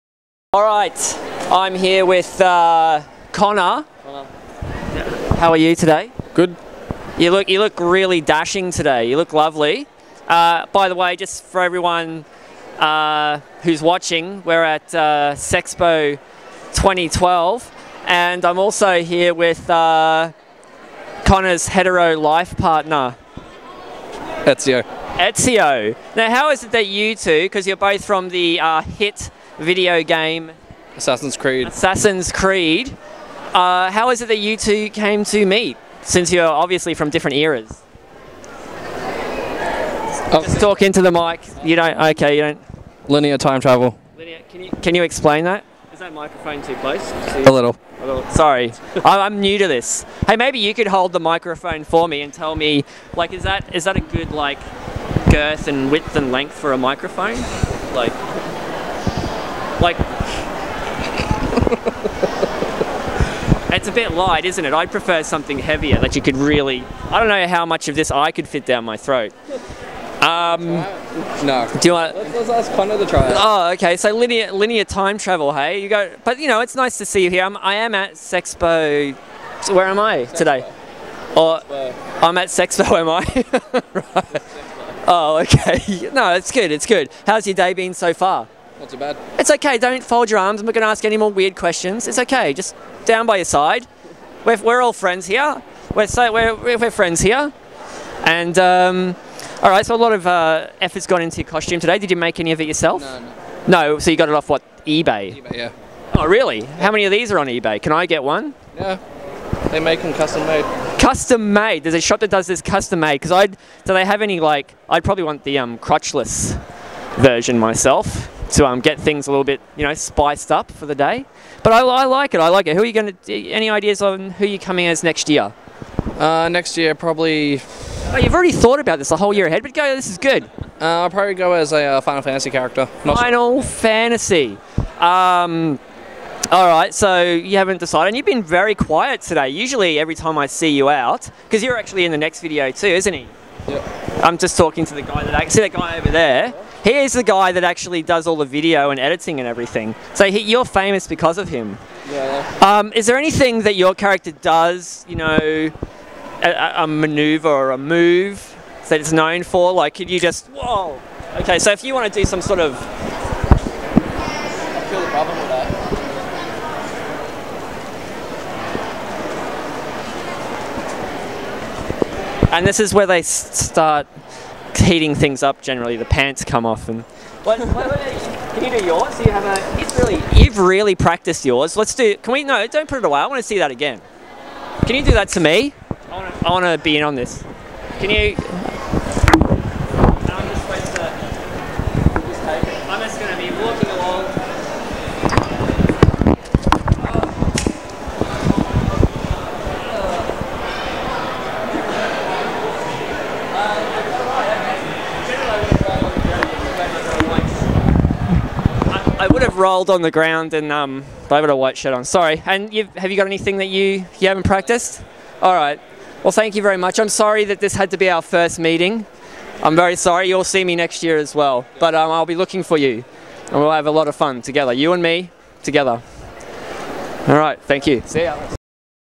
Assassin's Creed cosplay
Jay catches up with Ask Brisbane's regular, Connor (AKA Leon, AKA Zombie Superman), and his mate.